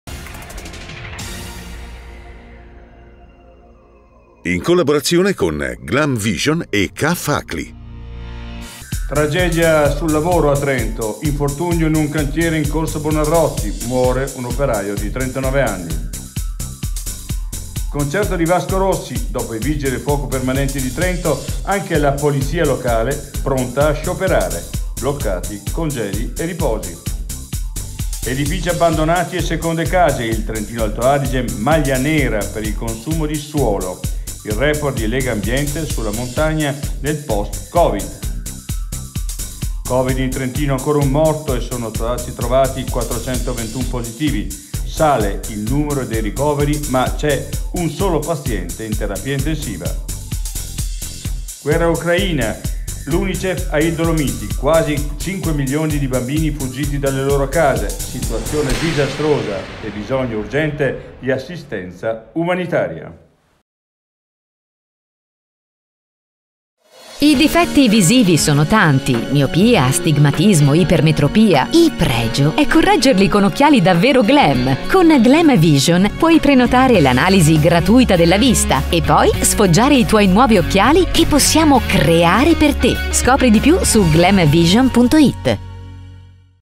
Tg de Il Dolomiti | 15.04.2022
Le notizie de Il Dolomiti di venerdì 15 aprile lette da Nereo Pederzolli con CAF ACLI (Trento) e GlamVision Ottica